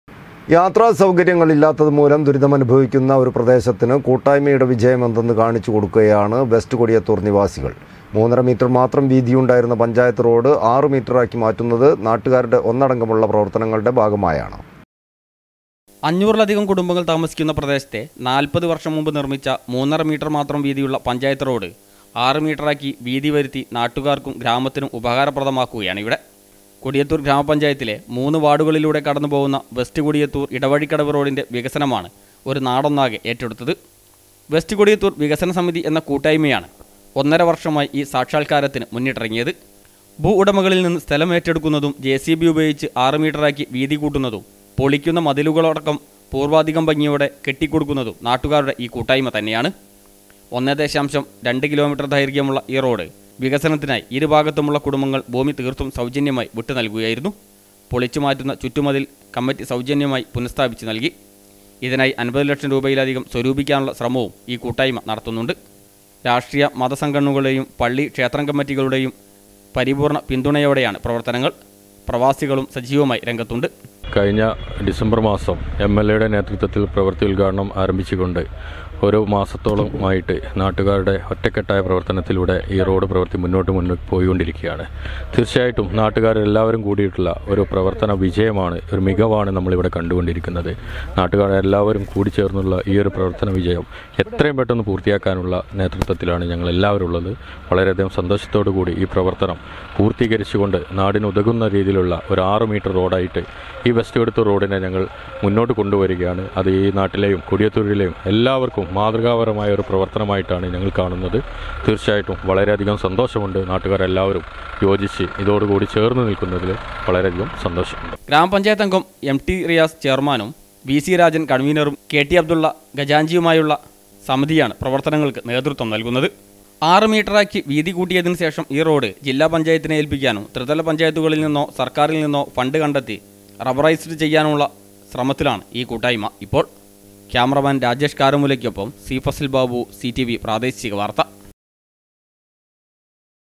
യാത്രാ സൗകര്യങ്ങൾ ഇല്ലാത്തത് മൂലം ദുരിതമനുഭവിക്കുന്ന ഒരു പ്രദേശത്തിന് കൂട്ടായ്മയുടെ വിജയമെന്തന്ന്
യാത്രാ സൗകര്യങ്ങൾ ഇല്ലാത്തത് മൂലം ദുരിതമനുഭവിക്കുന്ന ഒരു പ്രദേശത്തിന് കൂട്ടായ്മയുടെ വിജയമെന്തന്ന് കാണിച്ചു കൊടുക്കുകയാണ് വെസ്റ്റ് കൊടിയത്തൂർ നിവാസികൾ.
മൂന്നര മീറ്റർ മാത്രം വീതിയുണ്ടായിരുന്ന പഞ്ചായത്ത്‌ റോഡ് ആറ് മീറ്ററാക്കി മാറ്റുന്നത് നാട്ടുകാരുടെ ഒന്നടങ്കമുള്ള പ്രവർത്തനങ്ങളുടെ ഭാഗമായാണ്.